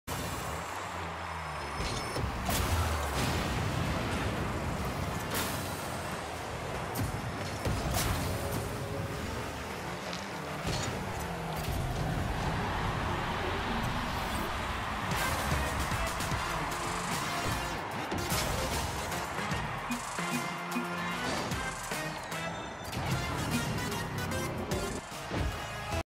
Clutch Teamate!
DeadMen-TNT came in clutch there, saving us from a loss!